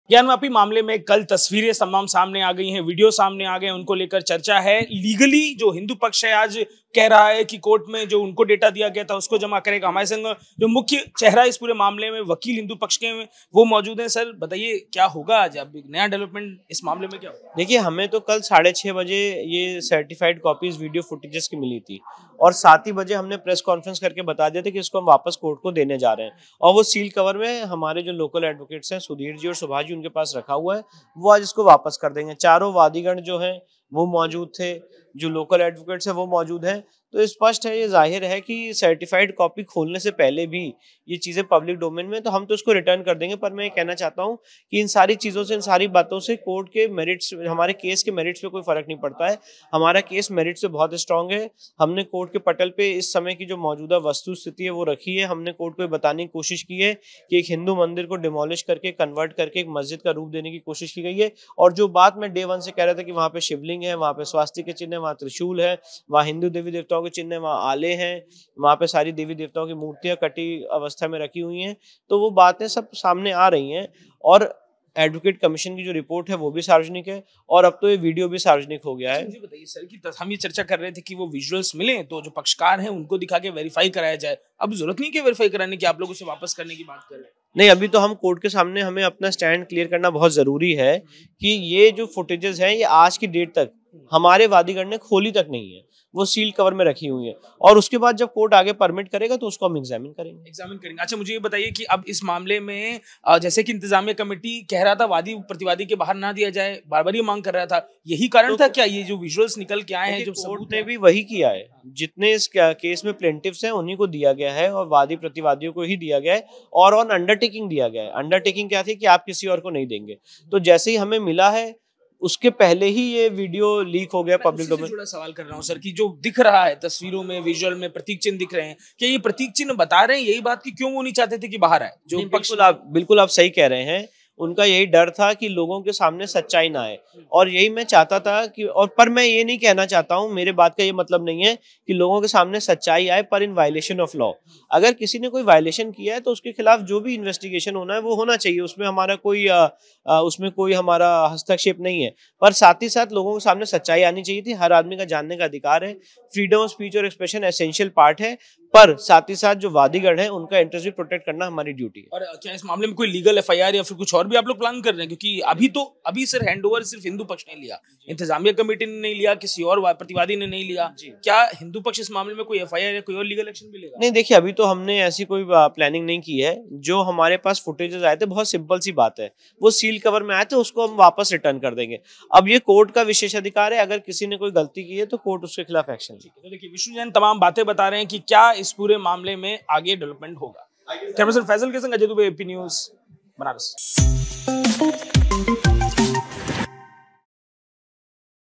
Gyanvapi Case: प्रमाणित प्रतियों को कोर्ट को वापस लौएगा वादी पक्ष, विष्‍णु जैन बोले- हमारा केस मजबूत
कोर्ट ने हमें जो लिफाफे में पेनड्राइव और दूसरा डाटा दिया था आज हमारे बनारस के वकील उसे कोर्ट के पास लेकर जाएंगे और बताएंगे कि देखिए हमारा लिफाफा सील है सूचना हमारी तरफ से बाहर नहीं है इंतजामिया कमेटी सूचनाओं के बाहर आने पर रोकटोक की बात करता था यह अब जी तस्वीरें सामने आई प्रमाण हैं कि हमारा वाद मजबूत होता है इस मामले में अब जुलाई में सुनवाई होगी.